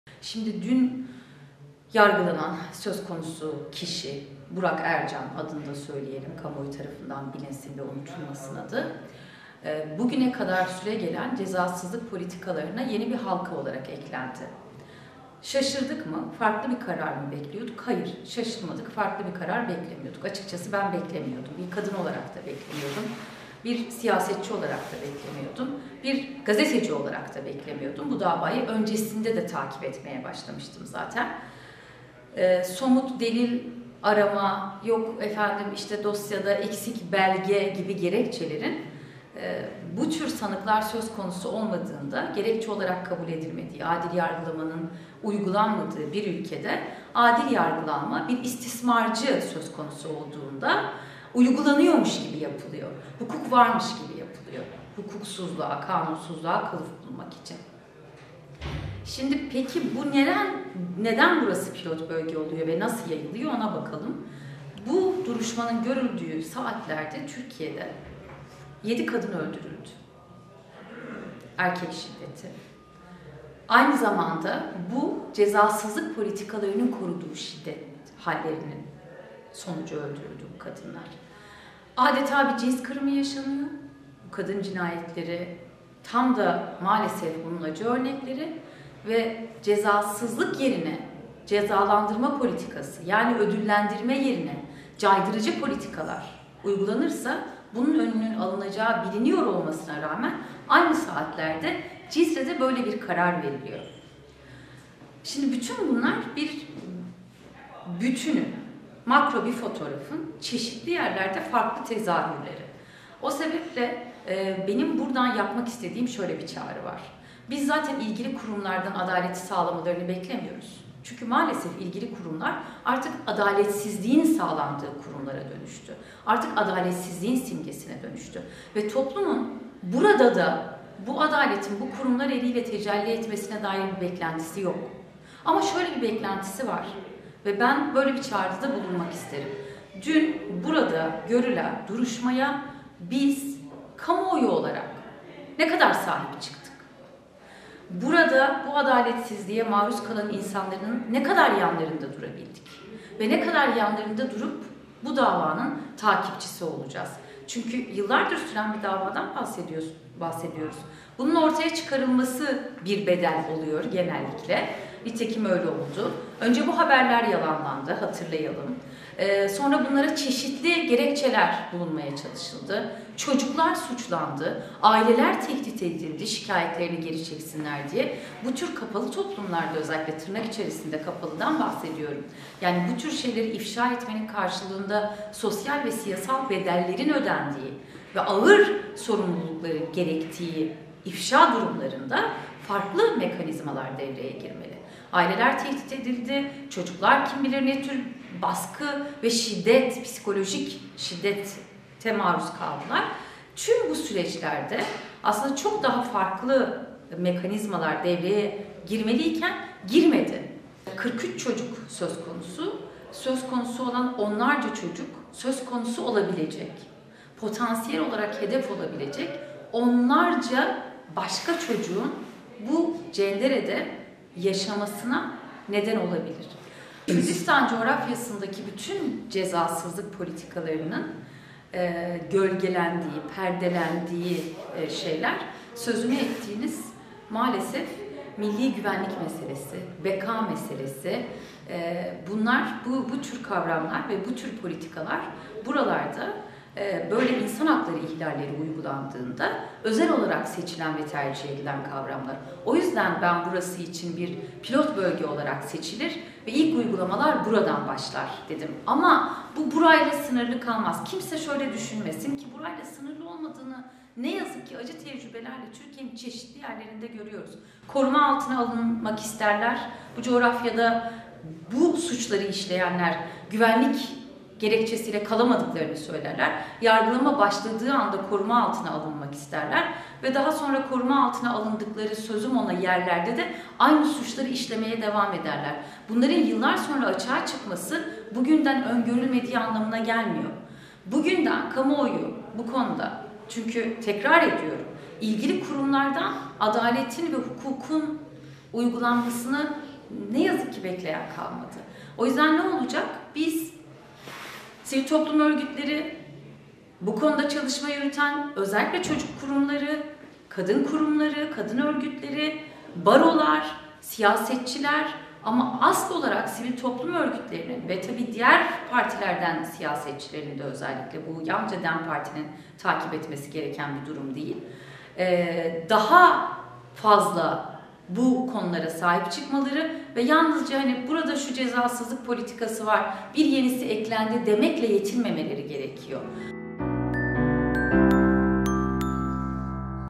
DEM PARTİ SÖZCÜSÜ DOĞAN: ADALET KURUMLARI ADALETSİZLİĞİN SİMGESİ OLDU
Cizîr’de 43 çocuğa yönelik istismar suçlamasıyla yargılanan Burak Ercan’a verilen cezaya tepki gösteren DEM Parti Sözcüsü Ayşegül Doğan, “Bu kurumlar adaletsizliğin simgesi oldular. Toplumun bu kurumlardan adalet beklentisi yok” dedi